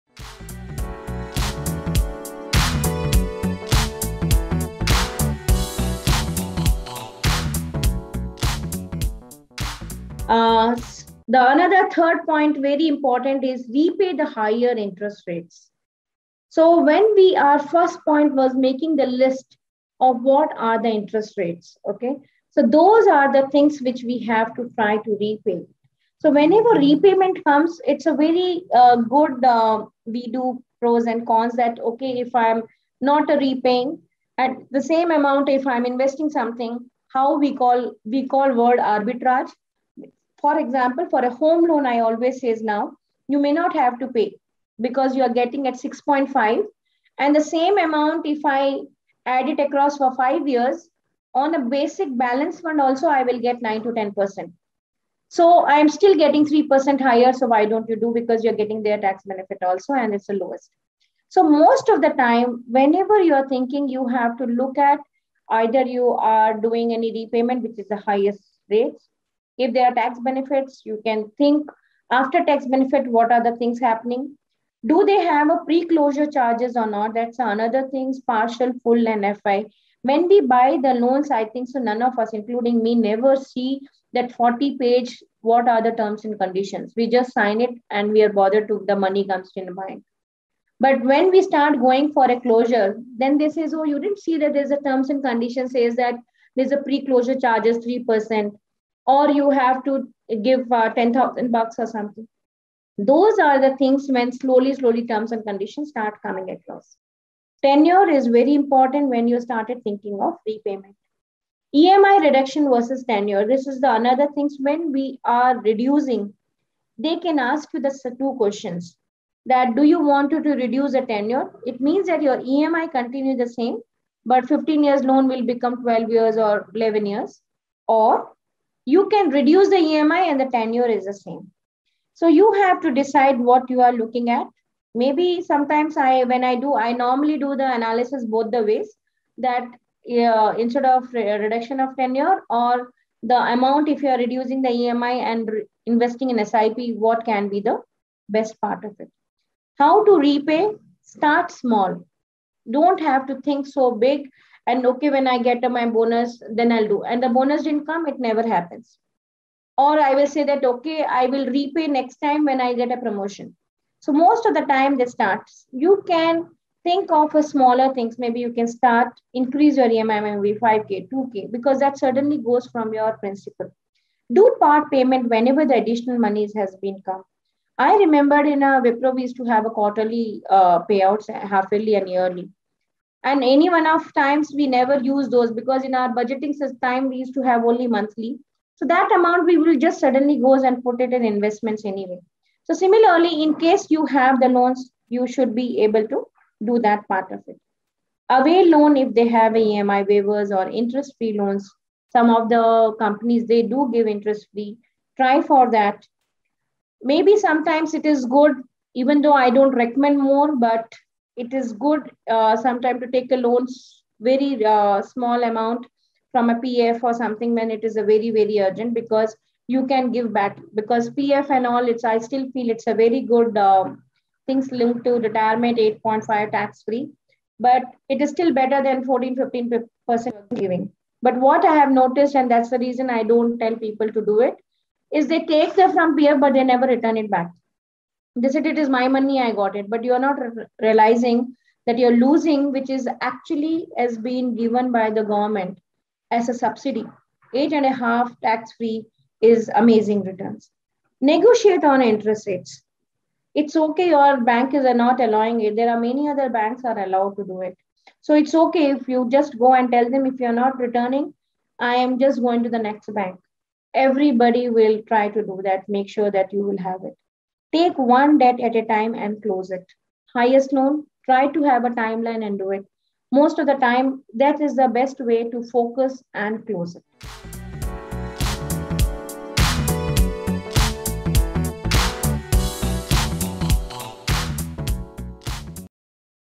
Repay Higher Interest Rate Debt | Steps in Debt Management | Right Horizons
Prioritizing the debt with the highest interest rate when you have many loans will save you more money and allow you to shift funds to other financial goals more quickly. Consider the following factors when preparing to repay debt: interest rates, tax benefits, and tenure vs. EMI reduction. This is a smart approach to cut down on the amount of interest you have to pay on your liabilities. To discover how to pay off debts, watch the entire video.